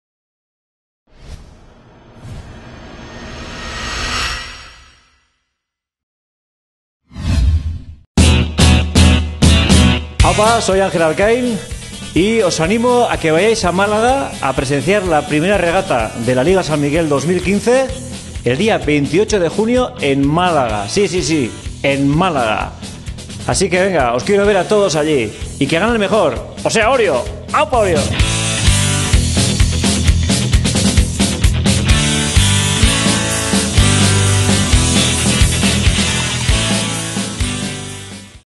Anjel Alkain también rema con la LigaSanMiguel
El humorista y presentador Anjel Alkain anima a acercarse el próximo 28 de junio a la Playa de La Malagueta